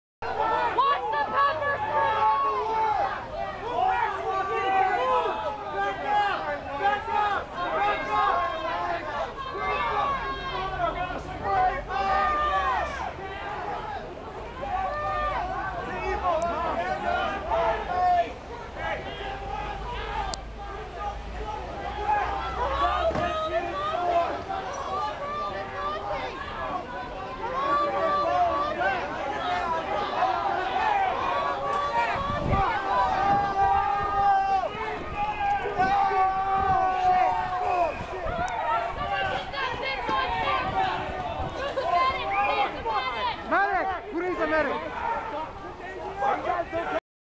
Cops pepper spray Occupy Seattle protesters at Shereton
Filmographer = Martin, an Occupy Seattle Occupier